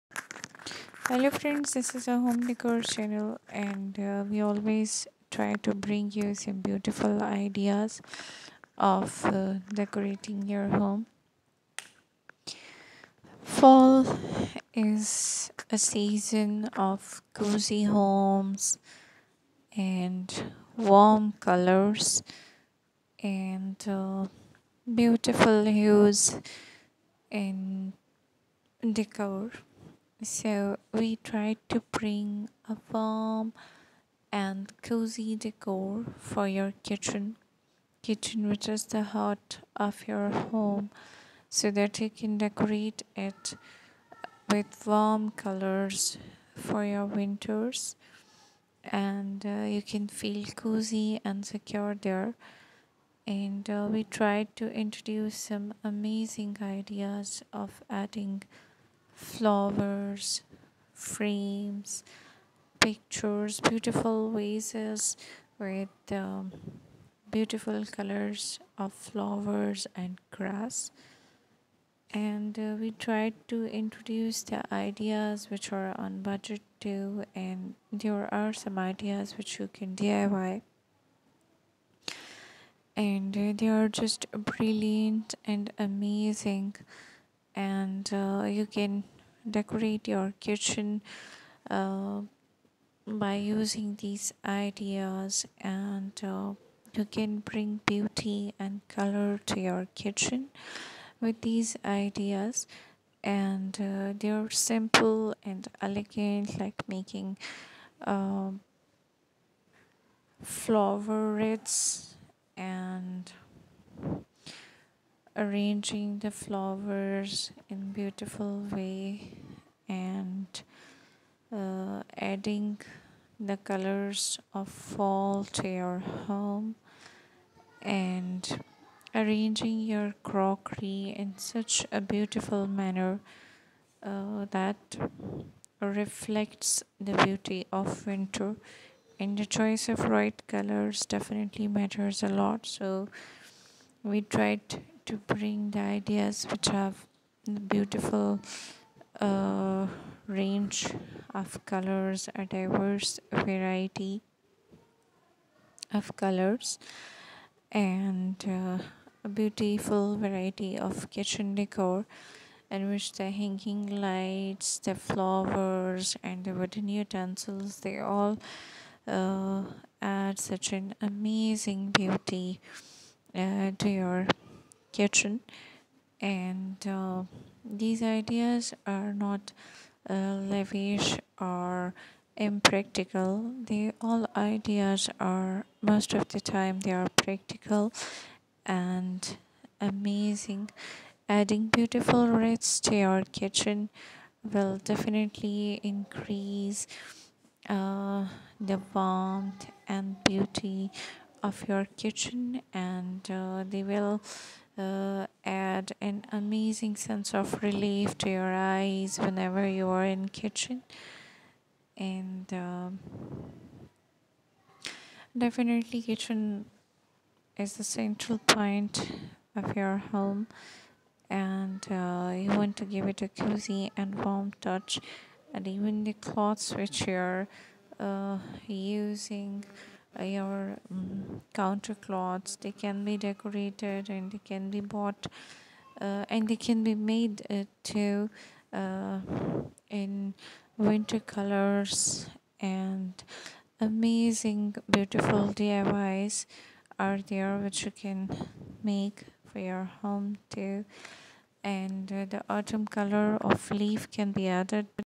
WonderfulFallKitchenDecorIdeas,40UniqueFallKitchenDecorIdeas, Fall Home Decor
This video is about vibrant and cozy colors of fall. These ideas can make your kitchen an amazing  place to work and enjoy in fall.

FALL KITCHEN DECORATE WITH ME 2022 | AUTUMN KITCHEN DECOR IDEAS
FALL 2023 DECORATE WITH ME 🍁 Part 1 | Fall Kitchen Decor
Crystal Tara
Jessica Giffin